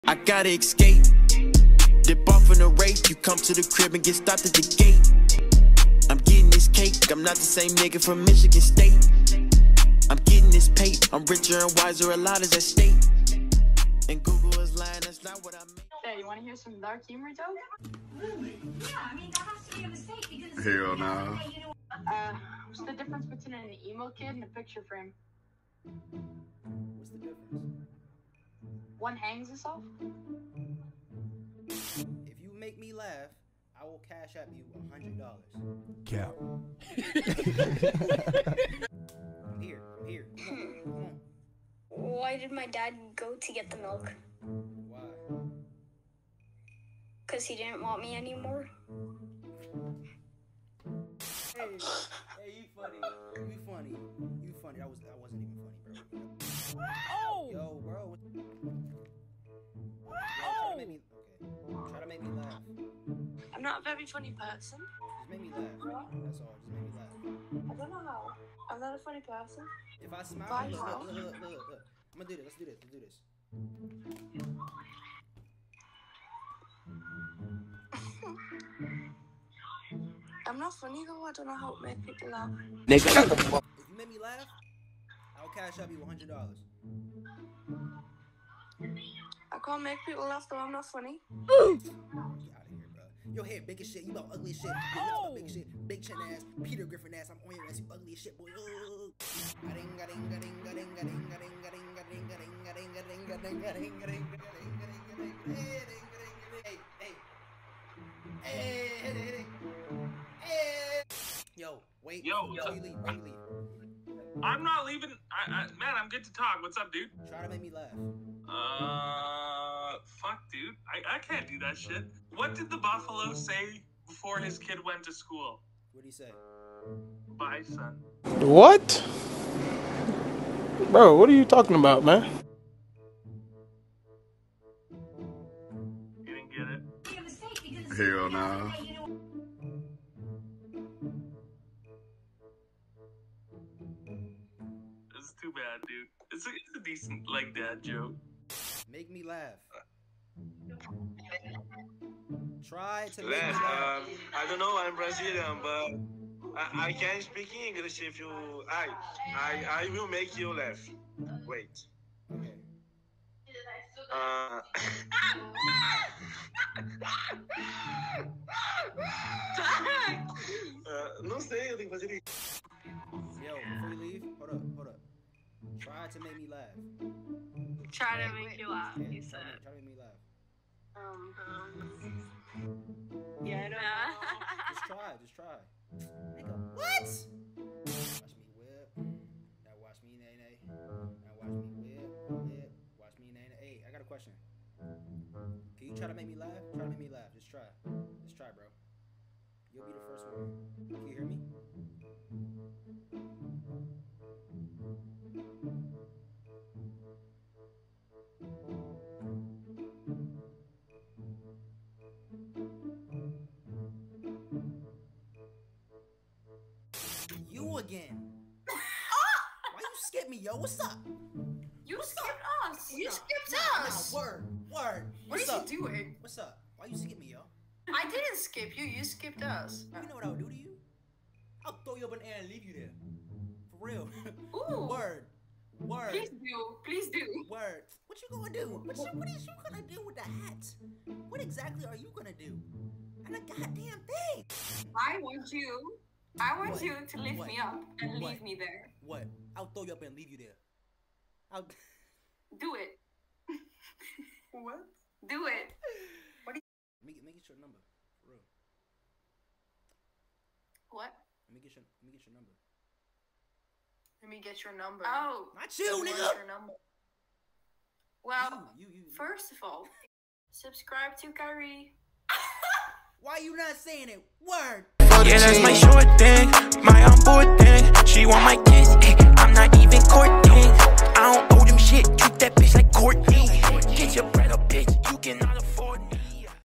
CAT FISHING GIRLS ON OMEGLE PRANK!!
Cat Fishing girls on OMEGLE prank and this happened...

I Love You Guys & I’ll See Y’all In The Next Video, Peace!!